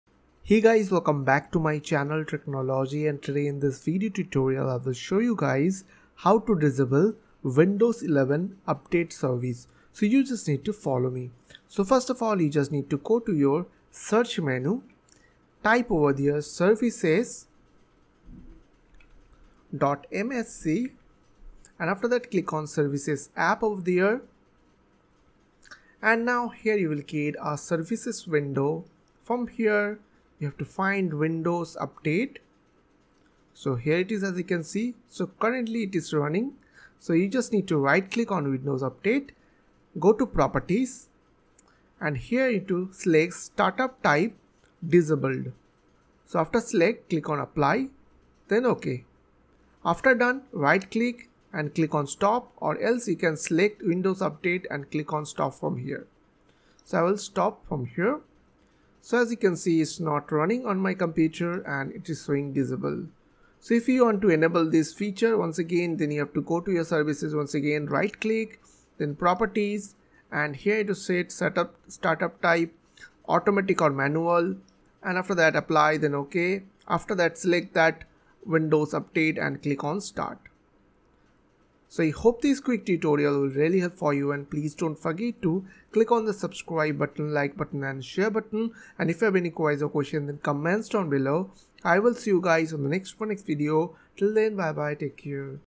How To Enable/Disable Windows 11 Update Service
In this video tutorial, I will show you guys how to enable or disable Windows 11 Update Service.

command: services.msc